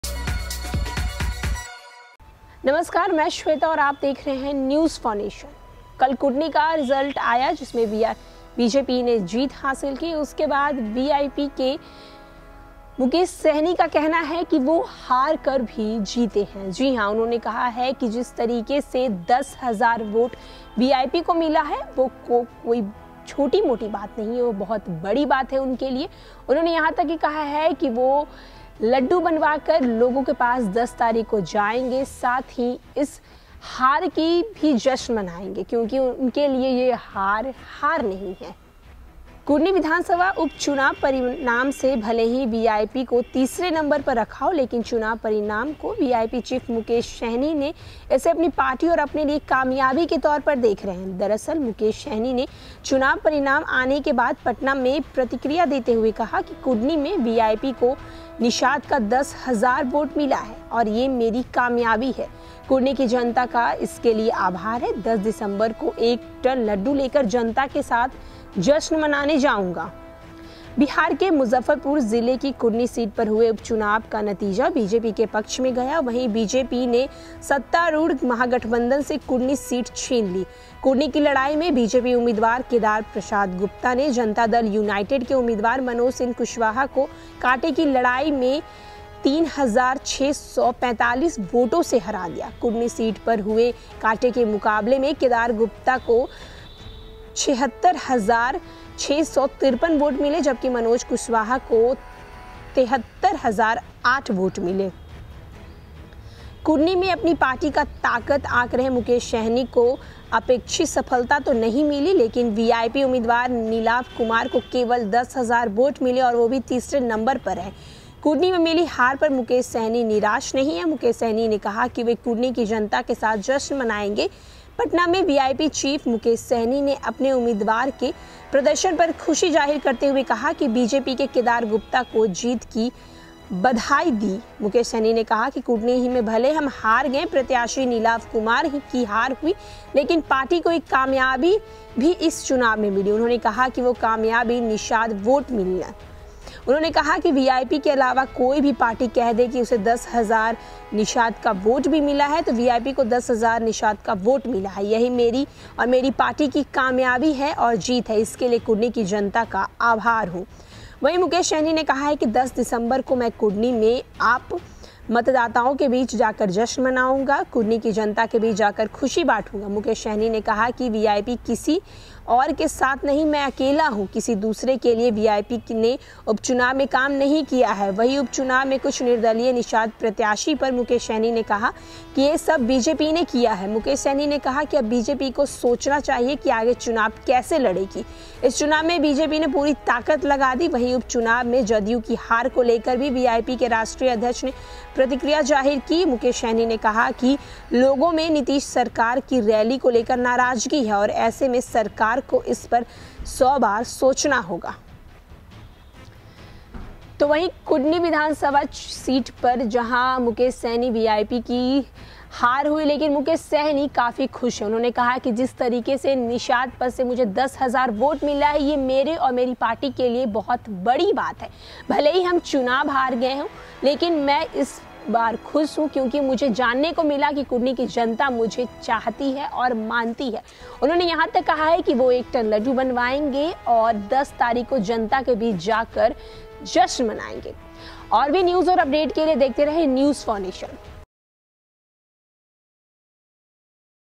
Mukesh Sahani कुढ़नी उपचुनाव में हार कर भी है खुश ,1 टन लड्डू लेकर जाएंगे यहां, देखिये |News4Nation
Mukesh Sehani कुढ़नी उपचुनाव में हार कर भी है खुश ,1 टन लड्डू लेकर जाएंगे यहां, देखिये |News4Nation